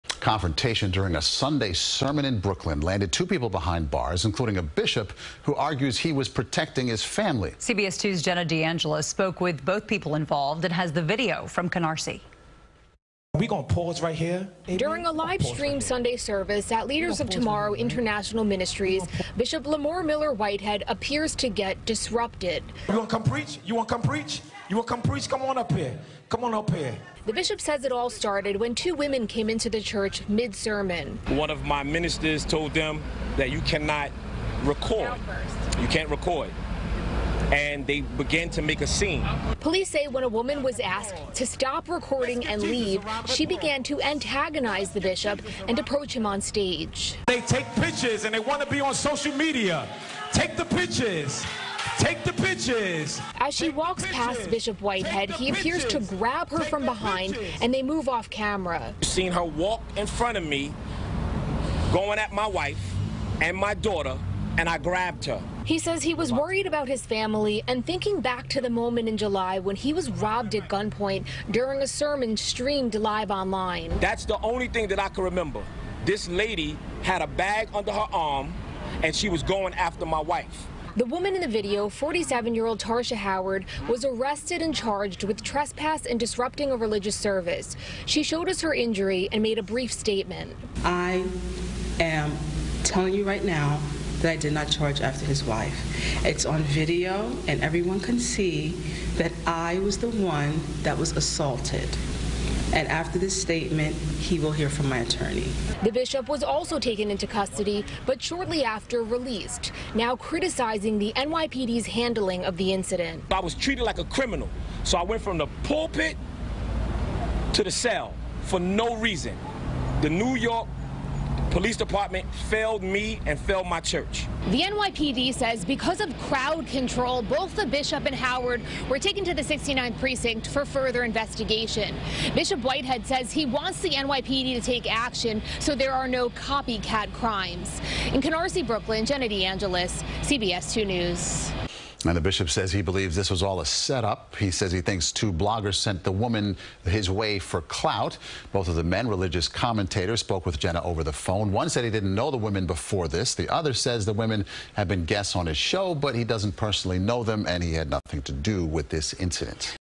Confrontation at Bishop Lamor Miller-Whitehead's sermon caught on video
A confrontation during a Sunday sermon in Brooklyn landed two people behind bars, including a bishop who argues he was protecting his family. CBS2's Jenna DeAngelis reports.